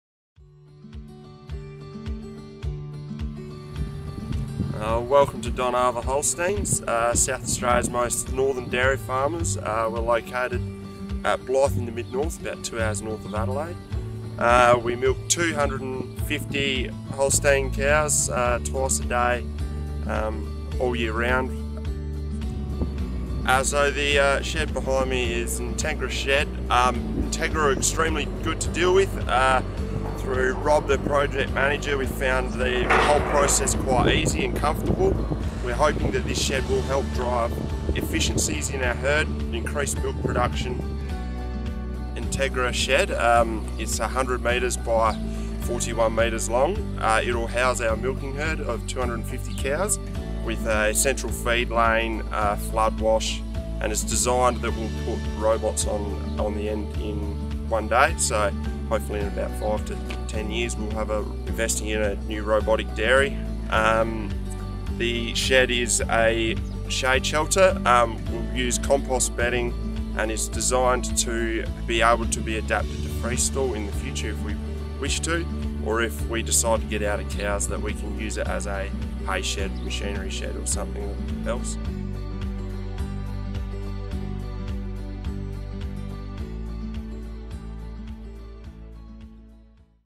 Inside South Australia's Most Northern Dairy Farm | Donava Holsteins
Donava Holsteins, South Australia's most northern dairy farm, is investing in modern infrastructure to improve efficiency, production, and future adaptability. Their newly built dairy facility, designed in collaboration with Entegra, is built to house 250 Holstein cows year-round, featuring a central feed lane and flood wash system for streamlined management.

A key aspect of the build is its future-proof design, allowing for easy integration of robotic milking systems within the next five to ten years. Currently operating as a shade shelter with compost bedding, the facility also has the flexibility to be converted into a freestall system or repurposed for hay or machinery storage if business needs change.

With automation and efficiency in mind, Donava Holsteins is positioning itself for long-term success in the evolving dairy industry. Watch how smart infrastructure choices are shaping the future of dairy farming in South Australia.